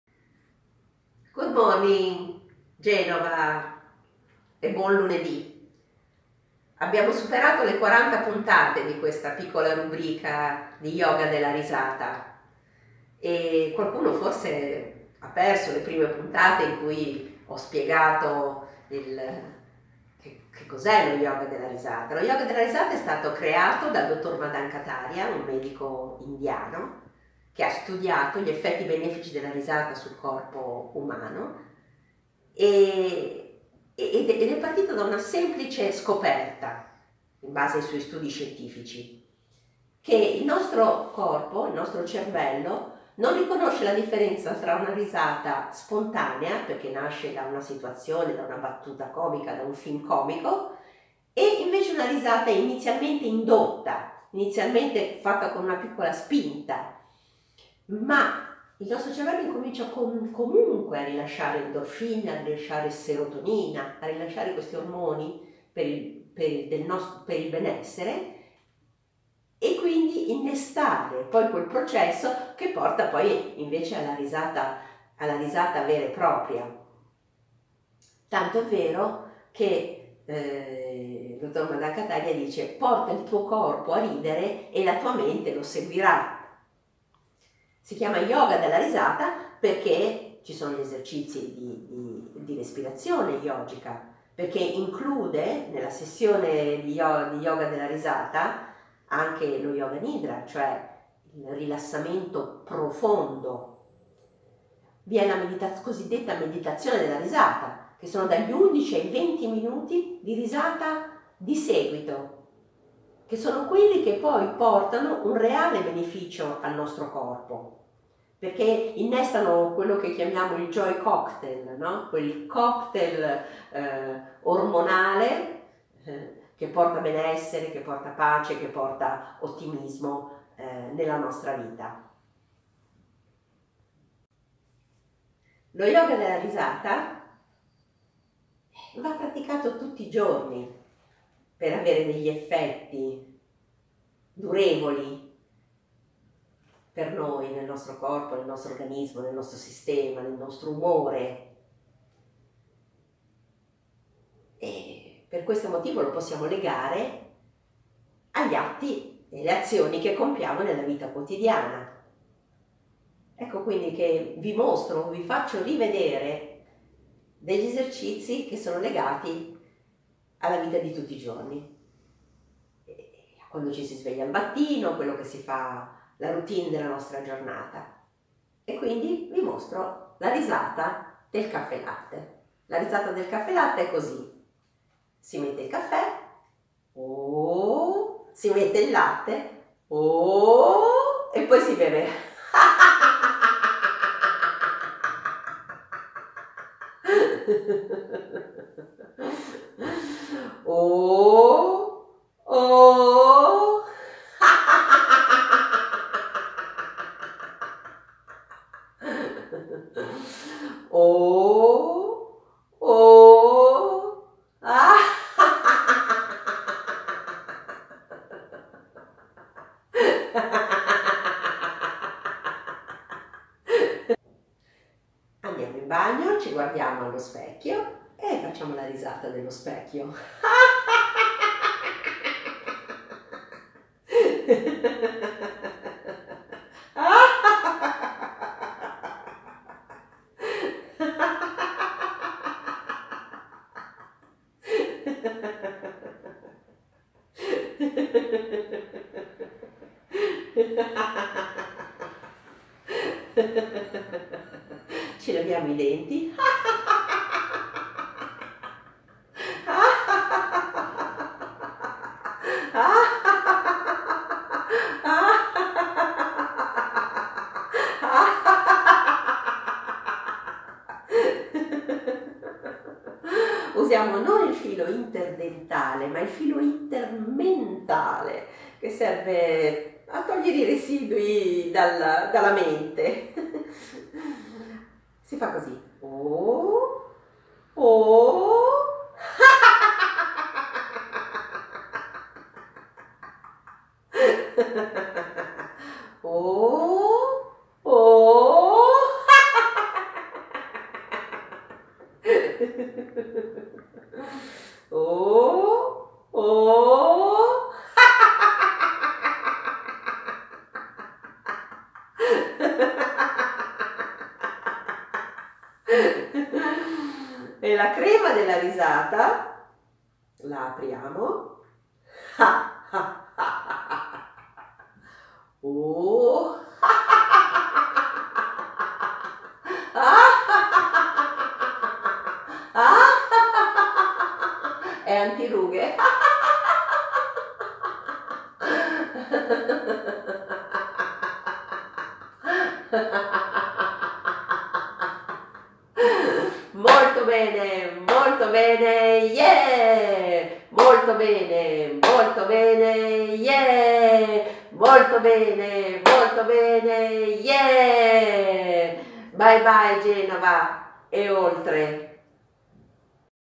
YOGA DELLA RISATA #43 con Giorgia Vigevano - Goodmorning Genova
Iniziamo la settimana con Giorgia Vigevano e lo YOGA DELLA RISATA focalizzando il nostro agire sulla risata indotta.